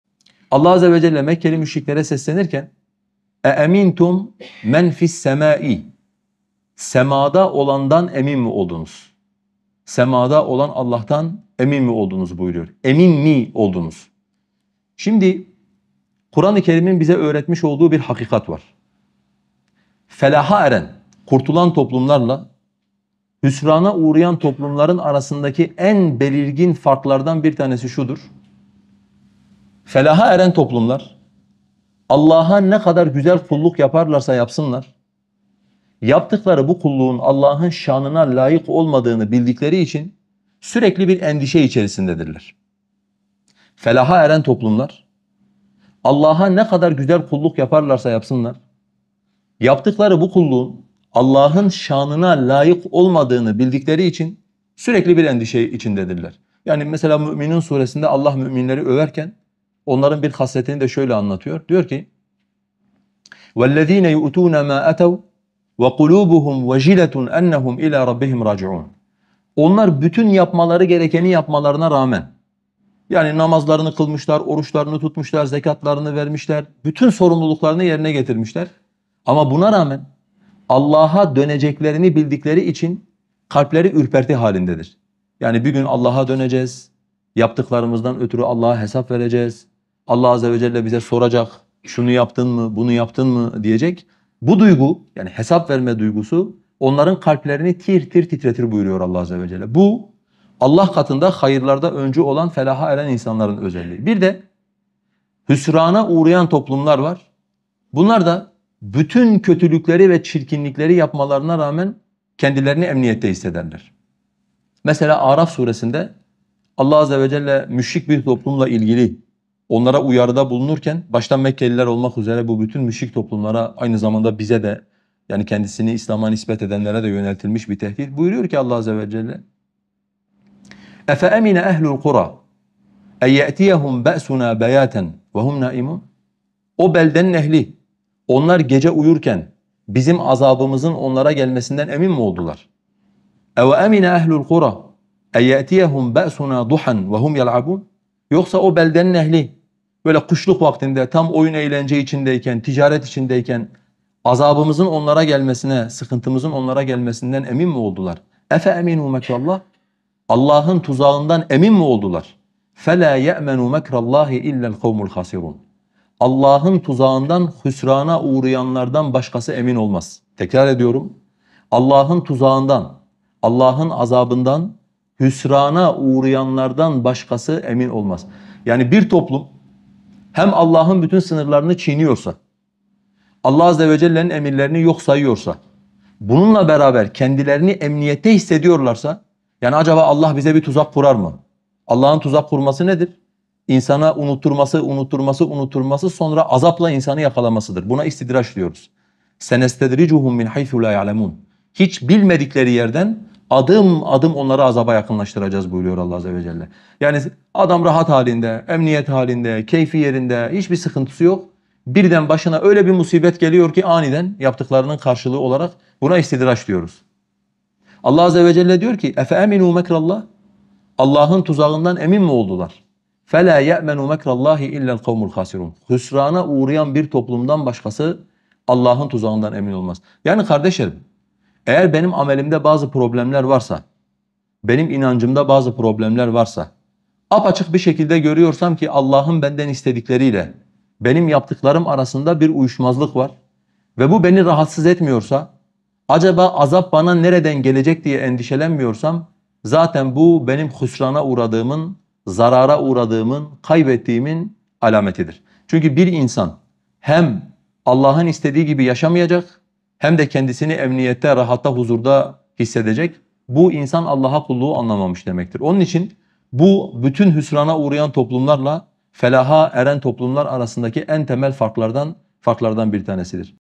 Helak Olanlar Ve Kurtulanlar | Halis Bayancuk Hoca
Uzun Hali İçin?

Mulk Suresi Tefsiri 7 | Gökte Olan Allah'tan Emin Mi Oldunuz? | Halis Bayancuk Hoca

ءَاَمِنْتُمْ مَنْ فِي السَّمَٓاءِ اَنْ يَخْسِفَ بِكُمُ الْاَرْضَ فَاِذَا هِيَ تَمُورُۙ
Gökte olan (Allah)’ın sizi yere geçirmeyeceğinden emin mi oldunuz? O zaman onun aniden sarsıldığını (görürsünüz). (67/Mülk, 16)

اَمْ اَمِنْتُمْ مَنْ فِي السَّمَٓاءِ اَنْ يُرْسِلَ عَلَيْكُمْ حَاصِبًاۜ فَسَتَعْلَمُونَ كَيْفَ نَذ۪يرِ
Gökte olan (Allah)’ın üzerinize taş yağdıran bir kasırga göndermesinden emin mi oldunuz? Uyarımın nasıl olduğunu bileceksiniz! (67/Mülk, 17)

وَلَقَدْ كَذَّبَ الَّذ۪ينَ مِنْ قَبْلِهِمْ فَكَيْفَ كَانَ نَك۪يرِ
Andolsun ki, onlardan öncekiler de yalanlamıştı. Nasılmış benim inkârım? (67/Mülk, 18)

اَوَلَمْ يَرَوْا اِلَى الطَّيْرِ فَوْقَهُمْ صَٓافَّاتٍ وَيَقْبِضْنَۜ مَا يُمْسِكُهُنَّ اِلَّا الرَّحْمٰنُۜ اِنَّهُ بِكُلِّ شَيْءٍ بَص۪يرٌ
Üstlerinde saflar hâlinde dizilmiş ve kanat açıp kapayan kuşları görmüyorlar mı? Onları, (hava boşluğunda) Rahmân’dan başkası tutmuyor. Şüphesiz ki O, her şeyi görendir. (67/Mülk, 19)